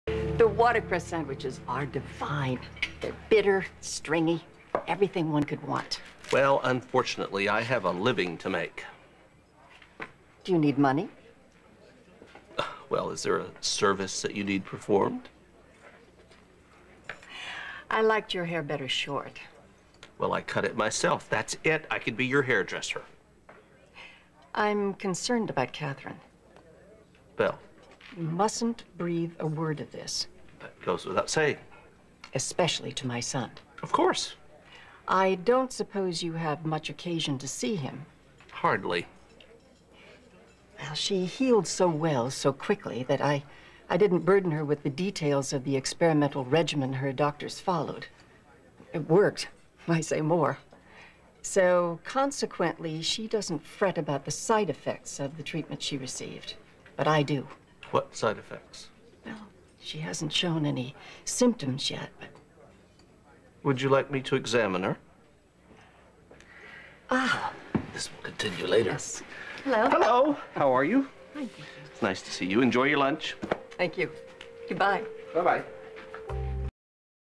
Tony meets with Helena
Tony exchanges pleasantries with Helena and asks how to be useful to her. Helena admits that Katherine Bell has undergone experimental treatment and Helena worries about the side effects. Tony offers to examine Katherine just as Katherine herself walks up to the table.